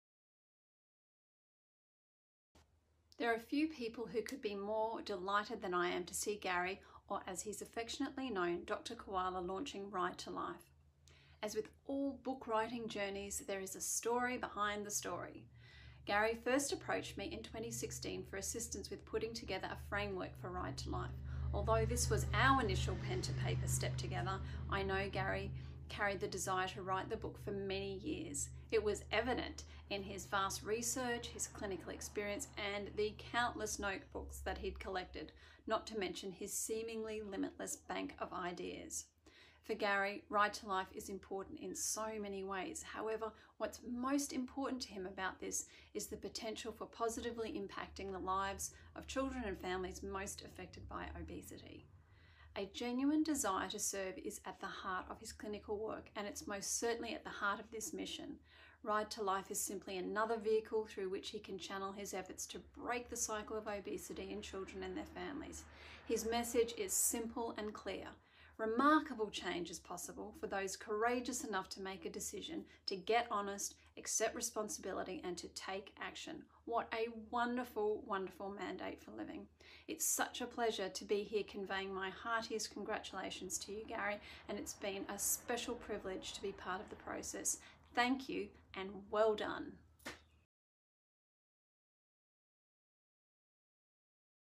Ms Macushla Collins, Content Coach from InCommZ, helping Dr Gary Leong launch his book Ride to Life
Ms Macushla Collins congratulating Dr. Gary "Koala" Leong on the launch of his Ride to Life book and program. Macushla was instrumental in helping Gary complete the first draft of his Ride to Life book. See her wonderful book  " 7-Day Book Blueprint - a step-by-step guide to planning, organizing and writing a book when you don't know where to start.